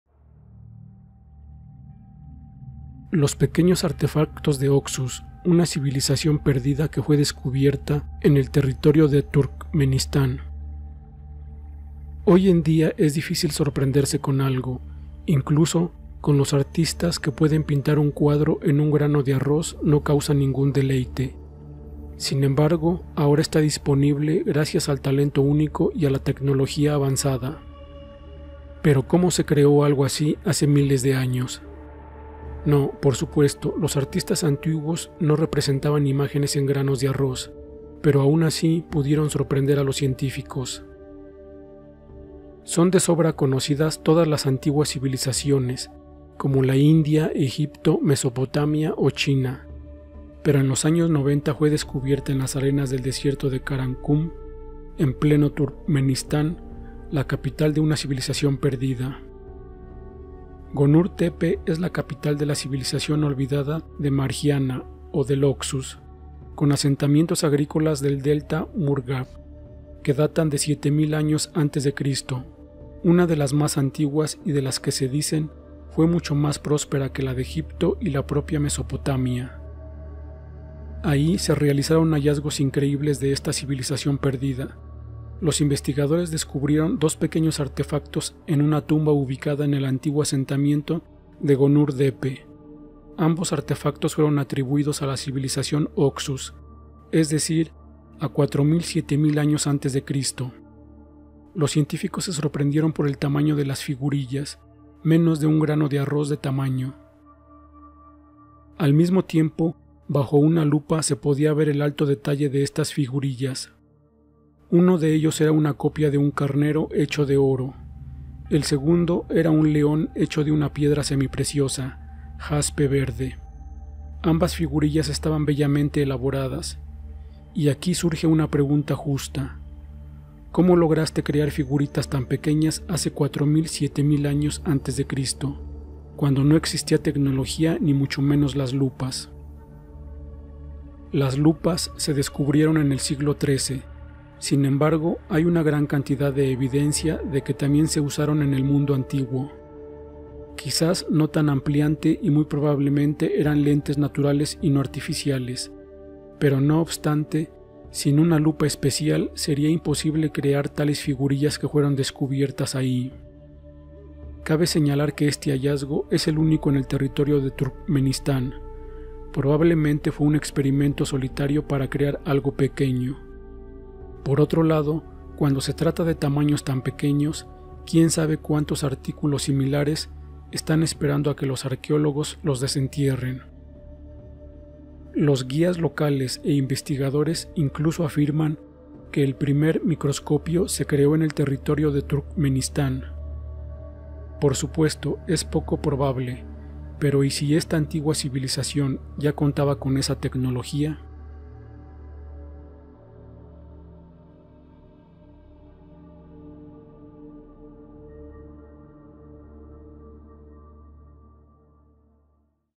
Los pequeños artefactos de Oxus una civilización perdida que fue descubierta en el territorio de Tur
Los pequeños artefactos de Oxus una civilización perdida que fue descubierta en el territorio de Turkmenistán